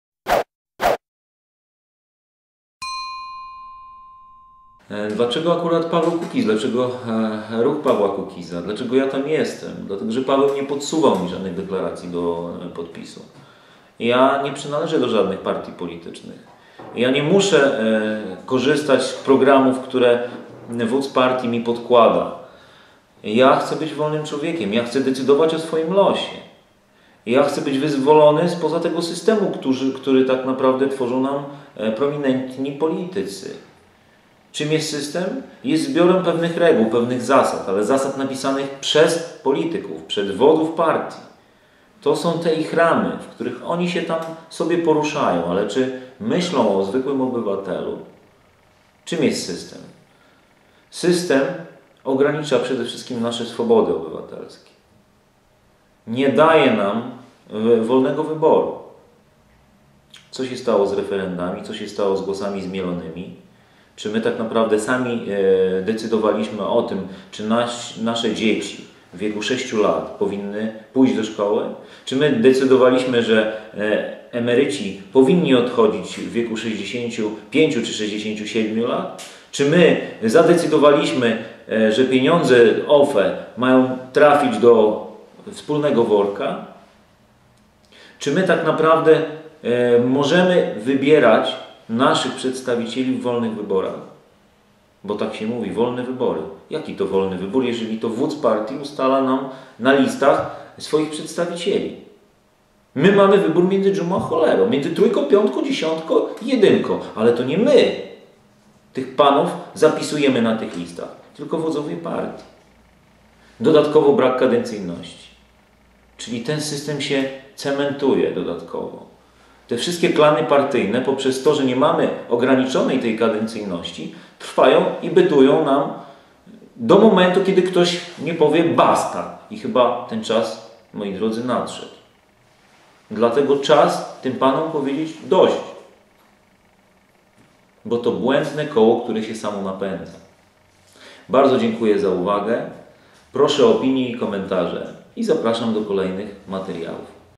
Adam Kiełczewski SAM NA SAM Dlaczego Paweł?
Adam Kiełczewski, kandydat na posła z ramienia ruchu Kukiz 15'. Numer 3 na liście w województwie Podlaskim. Okręg numer 24.